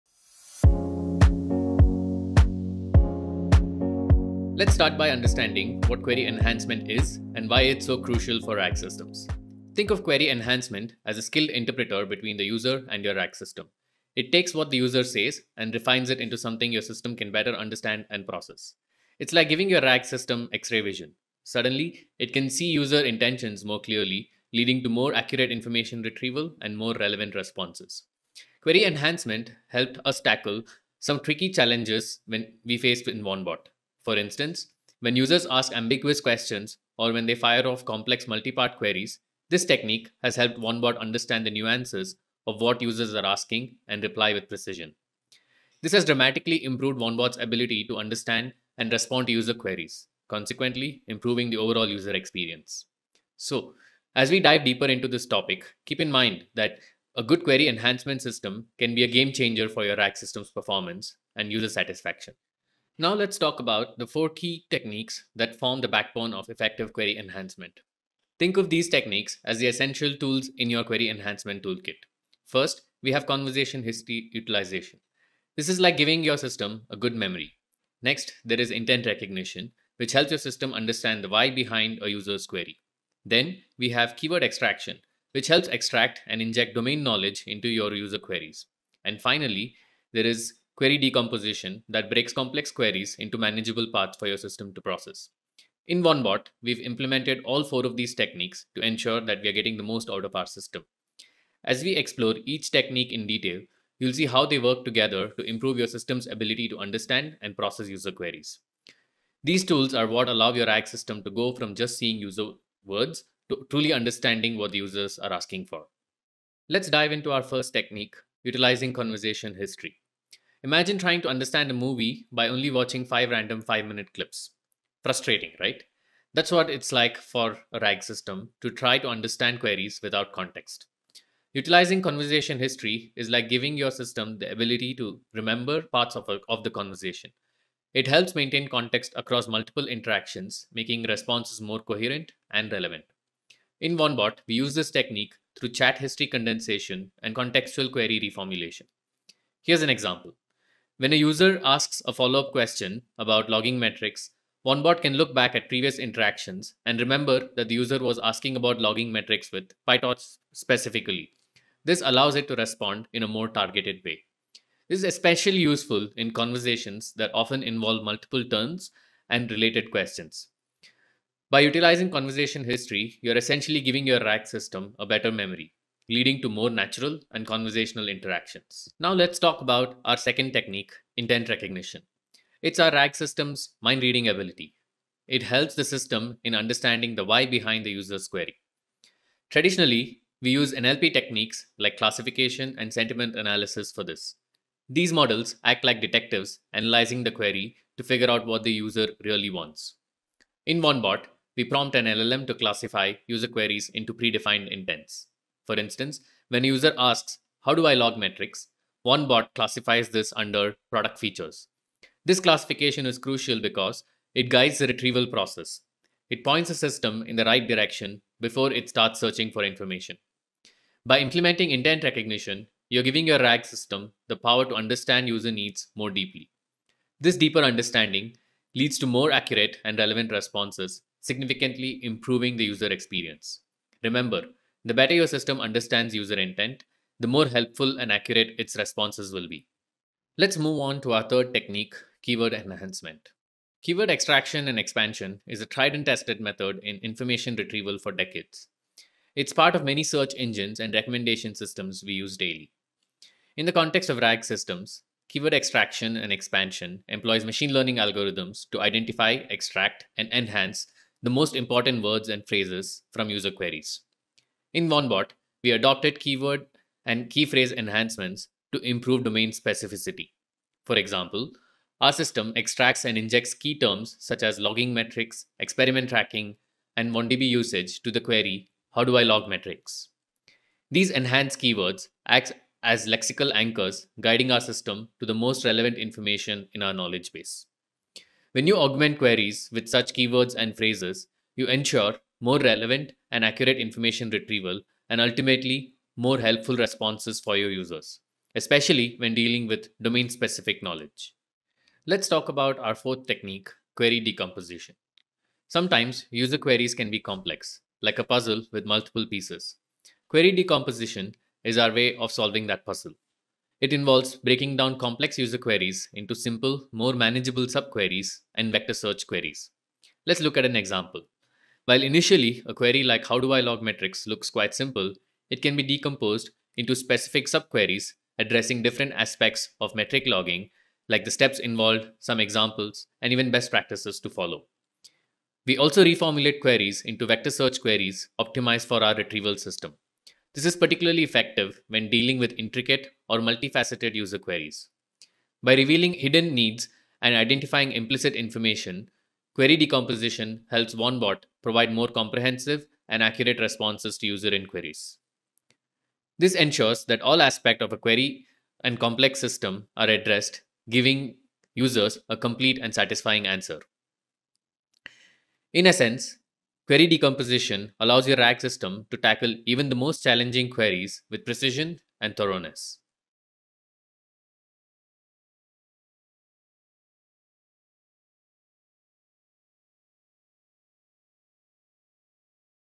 RAG++ course: 4 key techniques for query enhancement
This video is part of RAG++ : From POC to Production course, sign up for free

Query enhancement is essential for building sophisticated RAG systems that can effectively handle complex and nuanced user queries. By clarifying user intent, improving retrieval accuracy, and generating more relevant responses, query enhancement enhances the overall user experience and makes RAG systems more valuable and practical.

Check out the full course for more RAG lessons from experts from Cohere, Weights & Biases and Weaviate